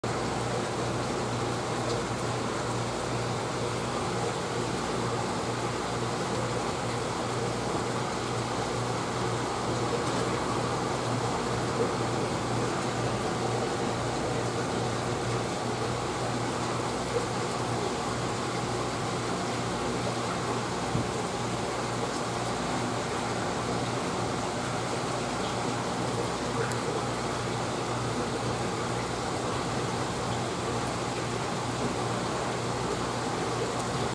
ヒブダイの捕食映像
生のシラスを食べるヒブダイ。
大きな体の割に、ほとんど食べないのでなかなか捕食シーンが取れない撮影者泣かせの魚。

The image on which a Scarus ghobban Forsskal eats the young fish of a sardine.